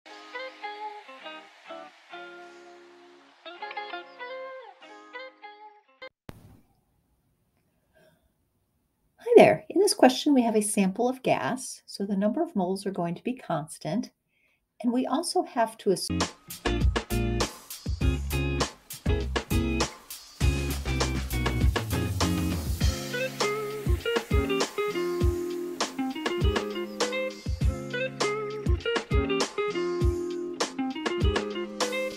4.00 L of a gas are under a pressure of 6.00 atm. What is the volume of the gas at 2.00 atm?
4.00 L of a gas are under a pressure of 6.00 atm. What is the volume of the gas at 2.00 atm?

DiscoverthePowerofGases:BenefitsandApplications